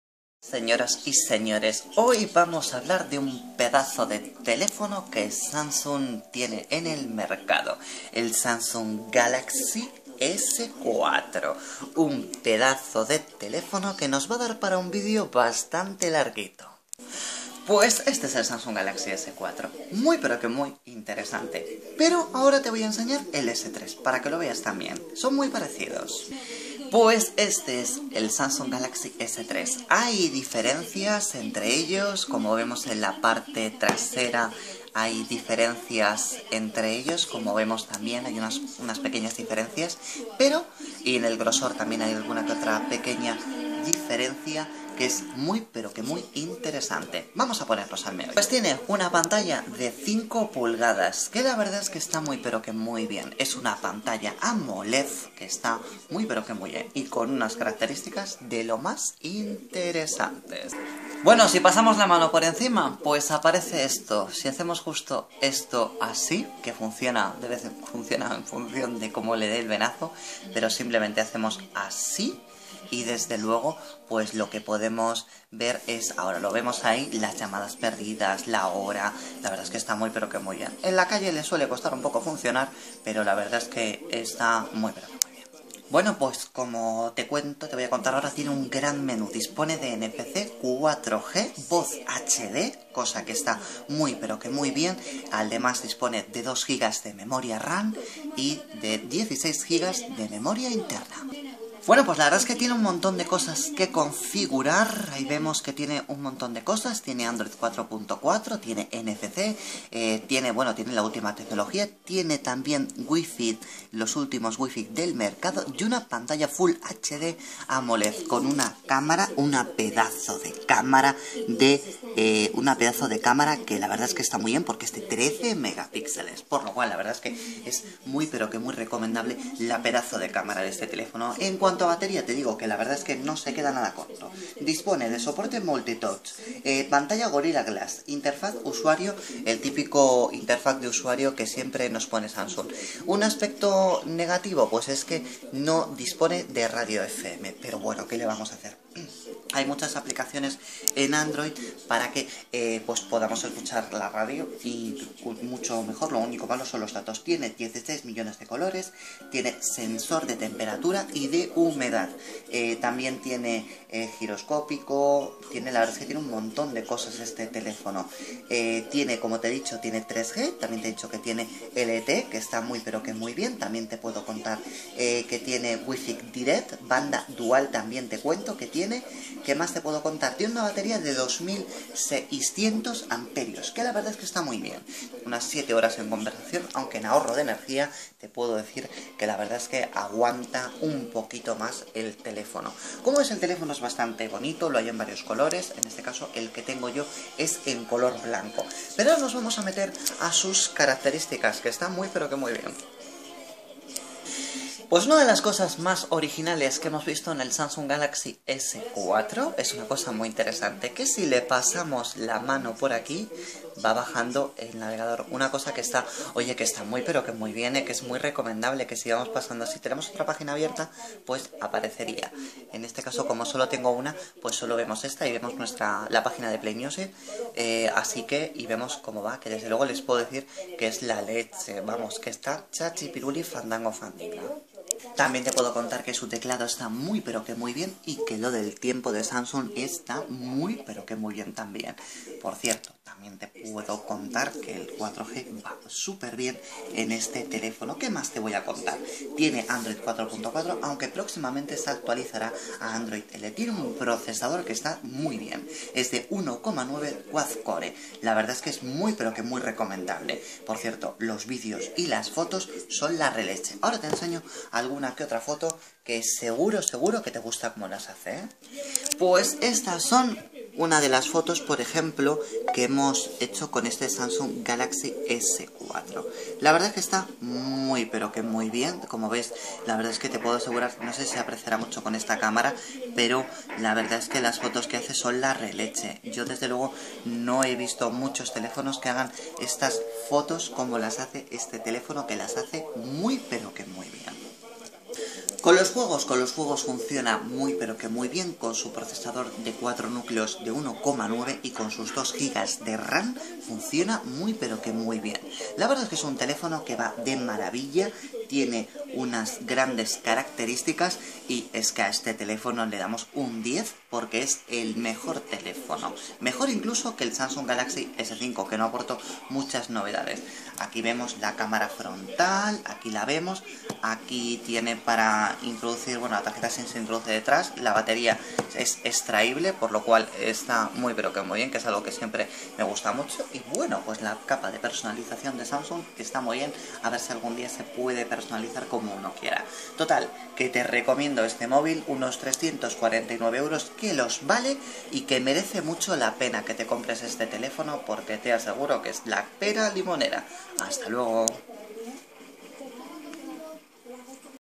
Samsung s4
Te hablo del Samsung s4 un móvil que mola mucho

Tiene 2 gb de ram y 16 de memoria  interna
Más un procesador  de 4 nucleos  de 1,9
(Terminal no es cedido por el fabricante )